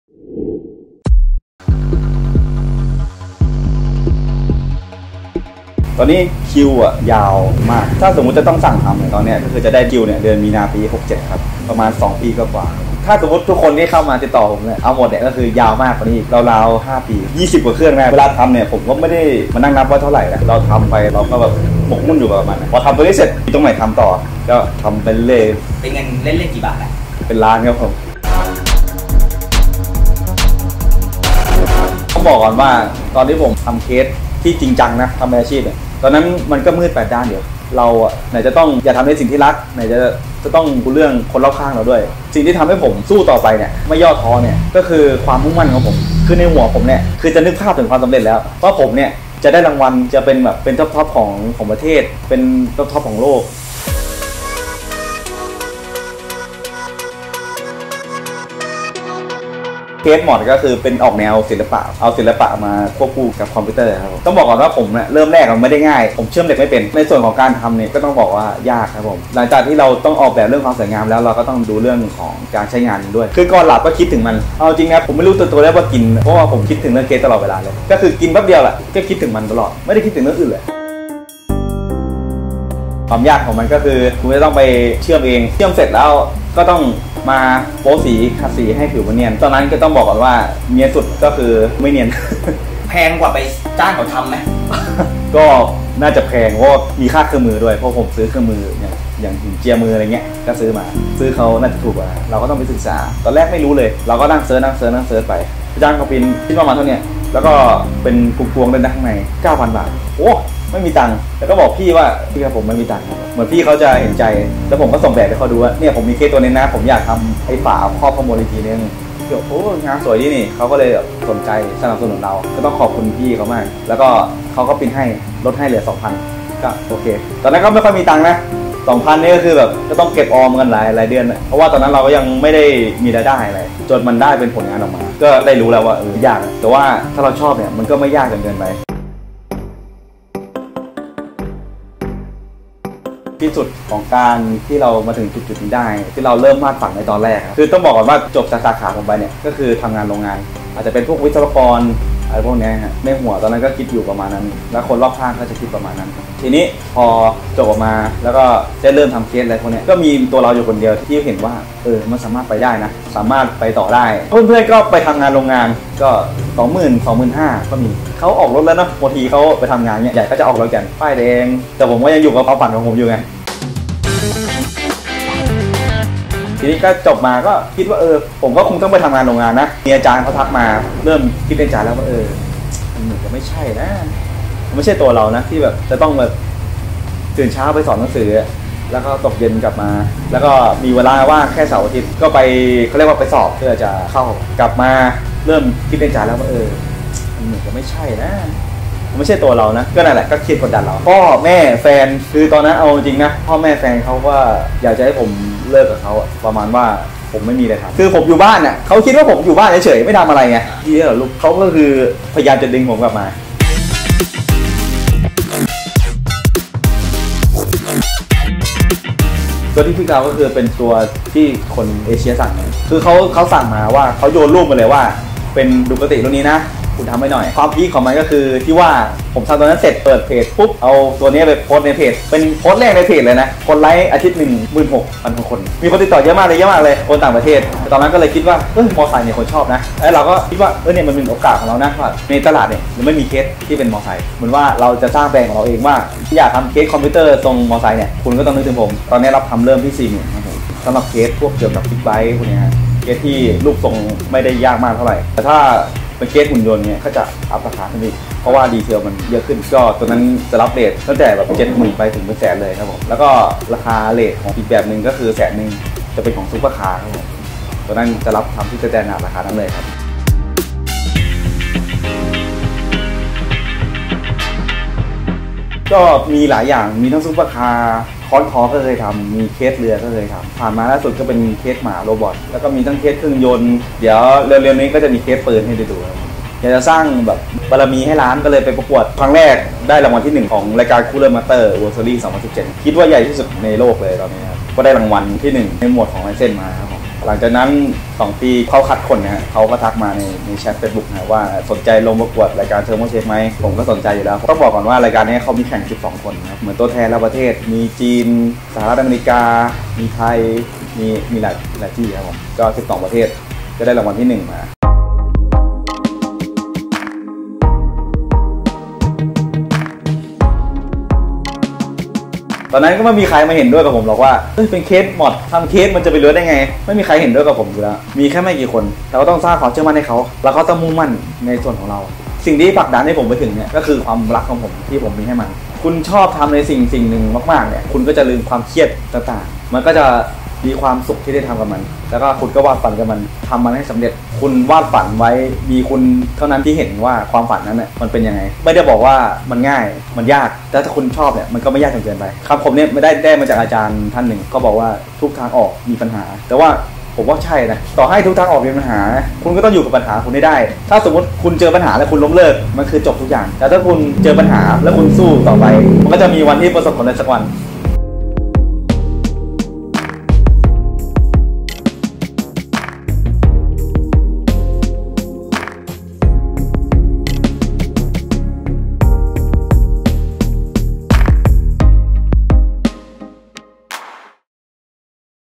"ปาล์มเคสม็อด" 5 ปีแต่งคอมหลักแสน/เครื่อง คิวยาว 2 ปีทำเงินเป็นล้านๆ
ผู้หลักผู้ใหญ่ที่บ้านไม่เข้าใจ “เคสม็อด” คืออะไรที่จะดีกว่า “ข้าราชการครู”
บ้านแฟนคนรักสั่งให้เลิกลา “ม็อดเดอร์” อาชีพ “ศิลปะบนเคสคอมพิวเตอร์” วันๆ อยู่แต่บ้าน
เพื่อนๆ วิศวะจบไปทำงานถอยรถป้ายแดงขี่ขับ
แต่ไม่มีใครหยั่งรู้หมู่ดาว ปฏิทินไม่มีบอกวันสำเร็จ 5ปี “ปาล์ม-จักรพรรดิ ทองเหลือ” วันนี้ วันๆ หนึ่งแต่งเคสคอมครั้งหนึ่งหลักแสน เป็นหนุ่มเนื้อหอมคิวจองยาว 2 ปี ไกลไปถึง 2567 ทำเงินเป็นล้านๆ บาทกับงานอิสระที่จะทำตอนไหนเมื่อไหร่ก็ได้
จากใครที่เห็นก็บอกไม่รอด! พ่อแม่บอกไปเป็นครูเหอะลูก! ตะเกียกตะกายพลิกตนสู่จักรพรรดิ “เทพเคสคอม” ระดับโลก เพราะไม่มีประโยชน์ที่เอาแต่รอและปล่อยฝันให้รอวันตาย และถ้าสู้...พรุ่งนี้มันต้องดีกว่า